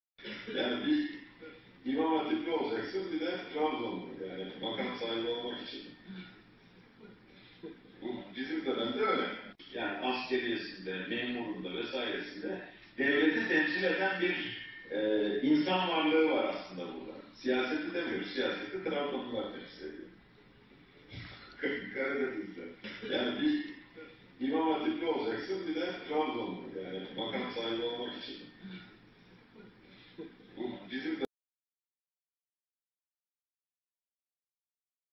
AKP’li Yücel Yılmaz: Makam sahibi olmak için ya ''imam hatipli'' ya da ''Trabzonlu'' olacaksın
AKP’li YücelYılmaz : Makam sahibi olmak için ya ''imam hatipli'' ya da ''Trabzonlu'' olacaksın